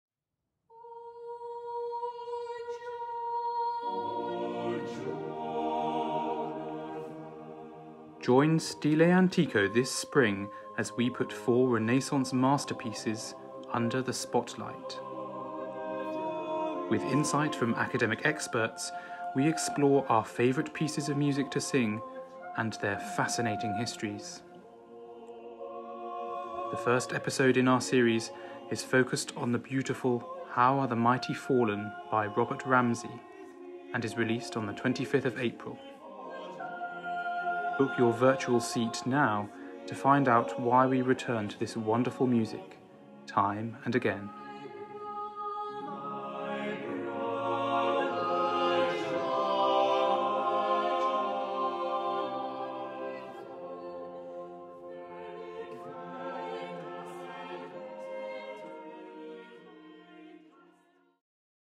Spotlights: first trailer
'Spotlights' is a new series of four on-demand videos from Stile Antico, each focused on a particular highlight from our repertoire. With the help of renowned academic experts, we will be putting each piece in context and exploring what makes it so special. Each episode culminates in a full performance of the featured work, and also includes a wealth of other music related to it, all newly filmed for this series.